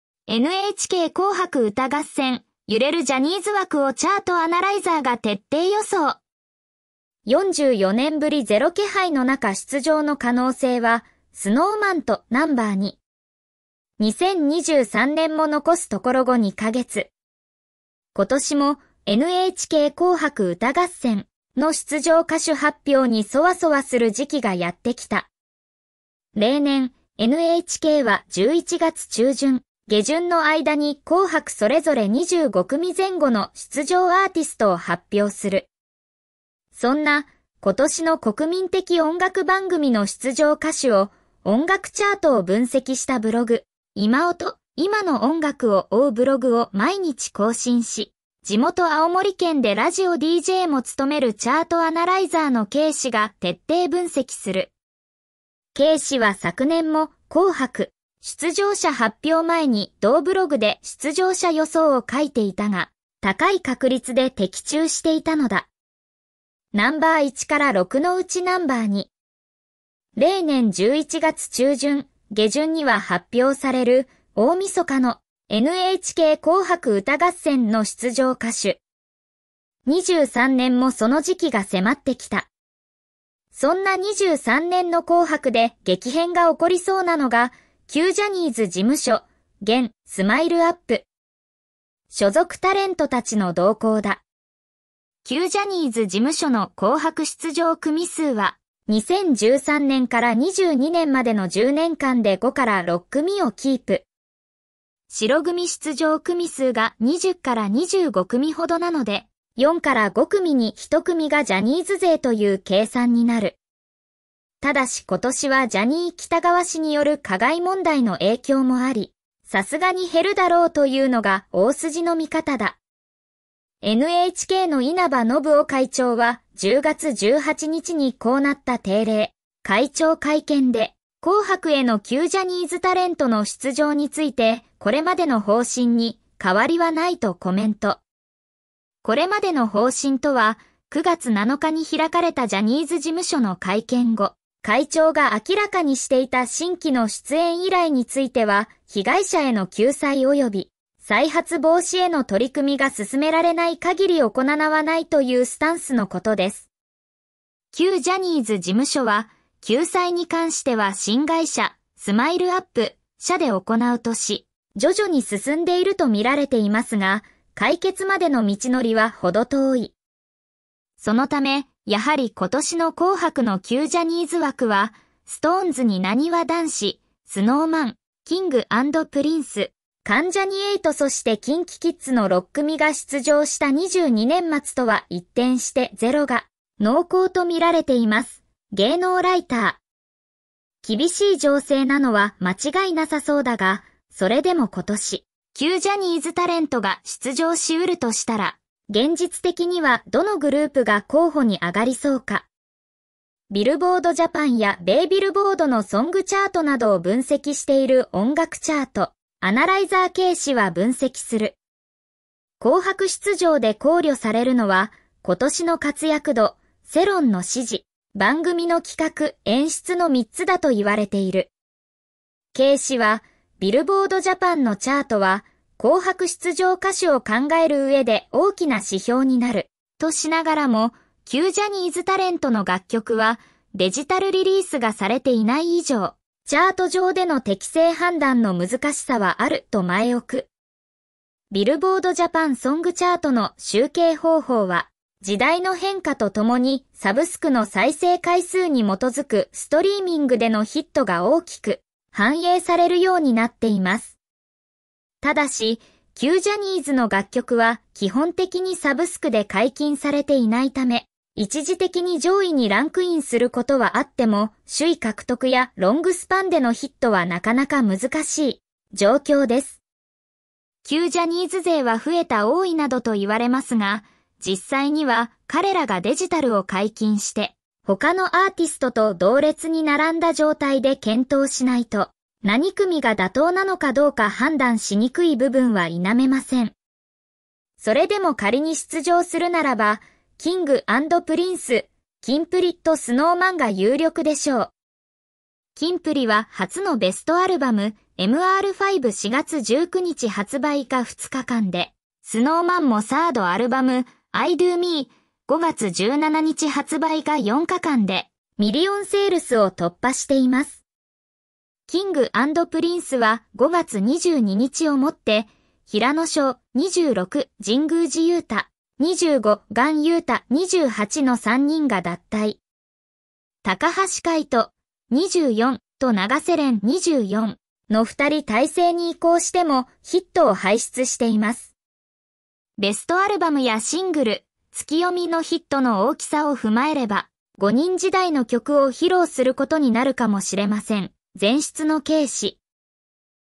NHK紅白歌合戦2024｜ジャニーズ枠が消滅危機!? SnowManの出演はどうなる？
NHK紅白歌合戦2024のジャニーズ枠に大注目！44年ぶりにジャニーズがゼロになる可能性が囁かれる中、チャートアナライザーが徹底予想します。今年の紅白にSnowManは出演するのか？その他のジャニーズグループの動向も含めて、最新情報を詳しく解説。ファン必見の分析動画です！